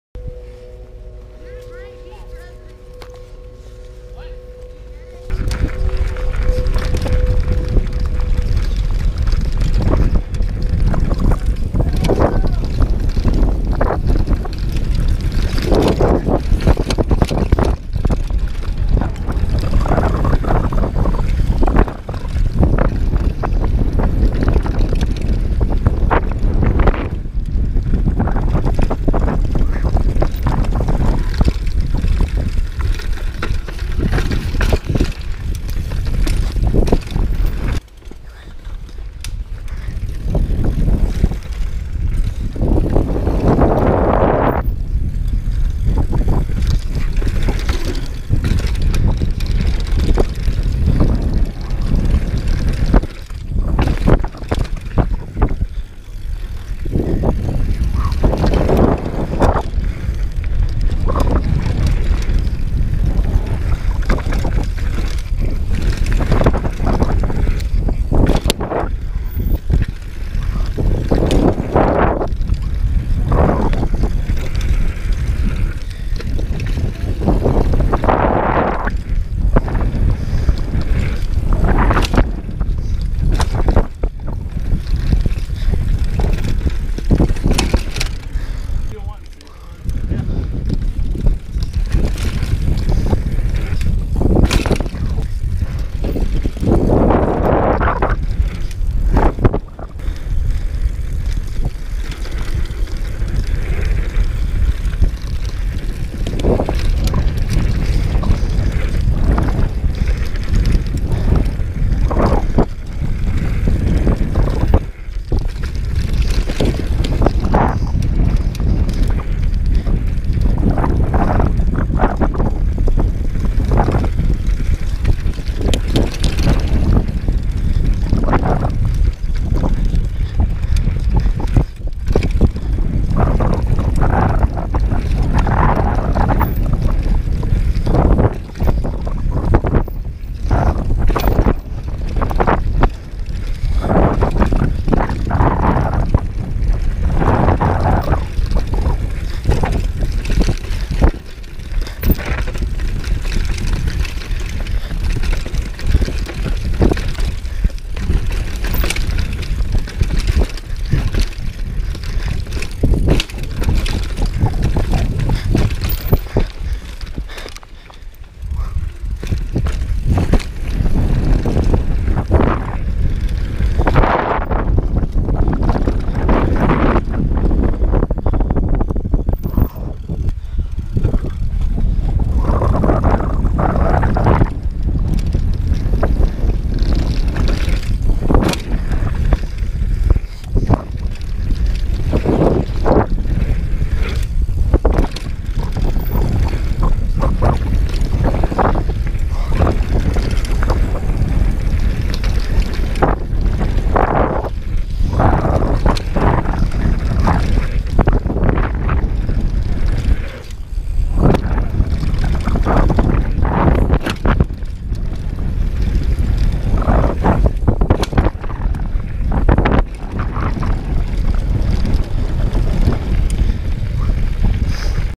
Shredding Beech Mountain with the new Gopro Gimbal!
Went slow because I was testing the new gimbal!
Trail: Burmese Python-Hellbender
Bike:Specialized Demo 8 Pro